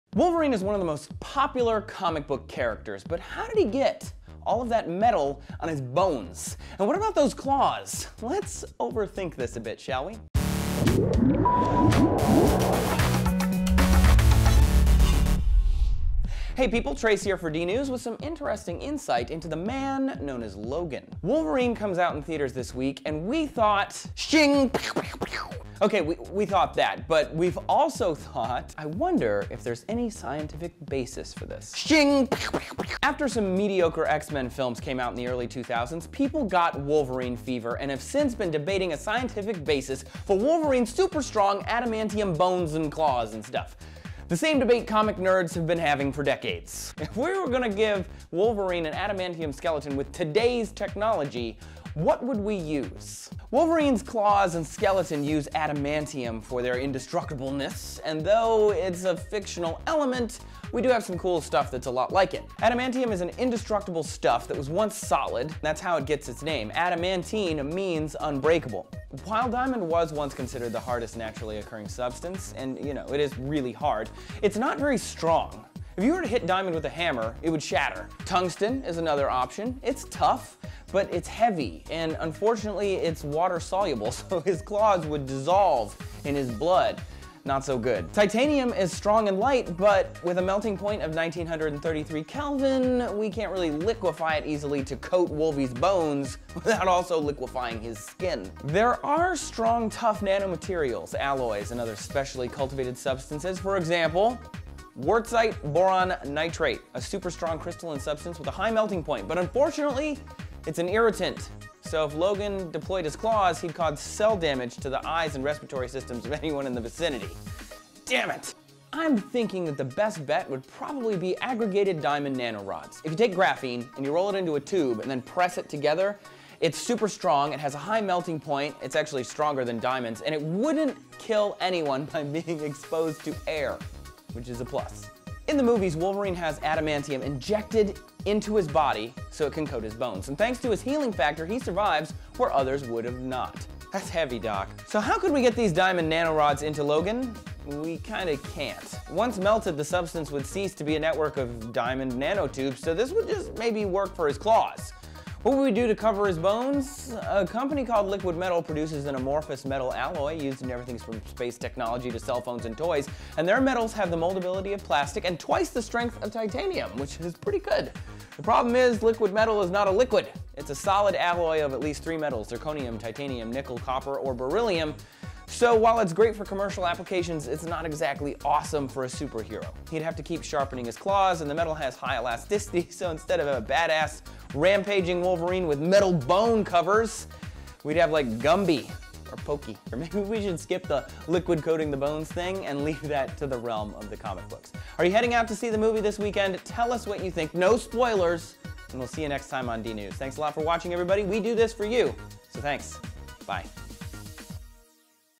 Real Life Wolverine Claws
"The Wolverine" is one of the biggest movies to hit theaters this summer. And along with his super healing powers, it's Wolverine's Adamantium skeleton and claws that makes him such a wicked superhero. That got Trace wondering: Is there anything like adamantium in real life?

Diamond no longer nature's hardest material
"The gemstone lost its title of the "world's hardest material" to man-made nanomaterials some time ago. Now a rare natural substance looks likely to leave them all far behind -- at 58% harder than diamond."

Ask a materials scientist to choose the strongest material, and you are likely to get a question instead of an answer. What kind of strong? Tensile strength? Toughness? Yield strength? Hardness? When it comes to materials, there isn't a single definition of strength but rather ways to measure certain characteristics.

What are Aggregated Diamond Nanorods?
"The least compressible material known, with a bulk modulus of 491 gigapascals (GPa) is aggregated diamond nanorods."

Giz Explains: What Is Liquidmetal?
"Apple has a new toy. It's a materials company called Liquidmetal, and everybody's talking! Problem is, nobody seems too sure what they're talking about. So, Liquidmetal: What is this stuff? And what does Apple want with it?"

"A revolution in metals has arrived. NASA, the California Institute of Technology (Caltech) and the U.S. Department of Energy united to help develop a new building material."